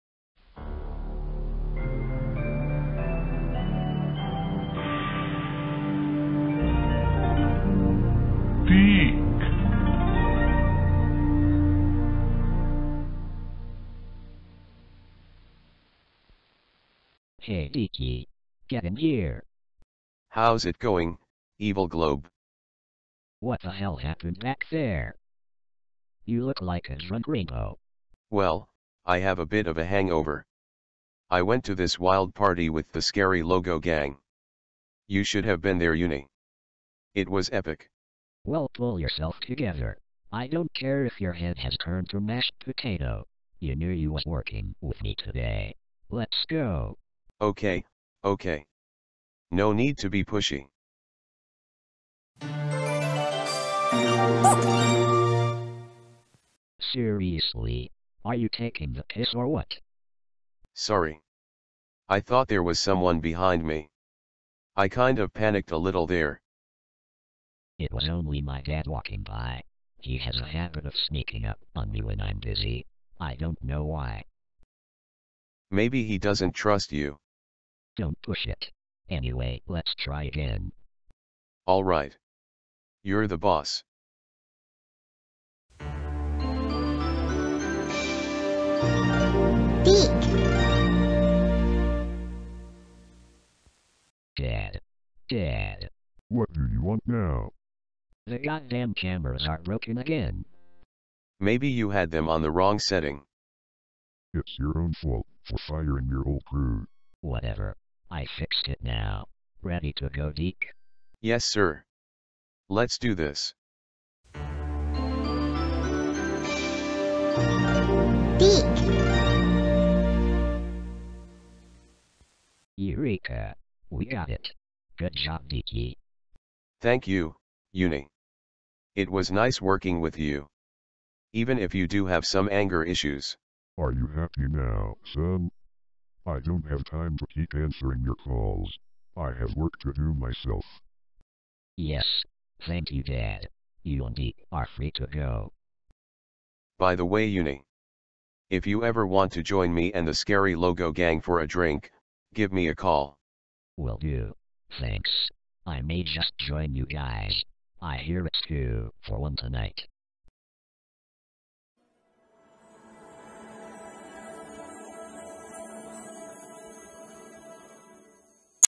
DiC Kid in Bed 1990 Blooper (Reupload)
The Evil Globe and his best friend the Silver DiC logo can't seem to get this take right.
Featuring special guest appearance from MCA TV (the Evil Globe's dad).

Reuploaded from the Evil Globe's old channel.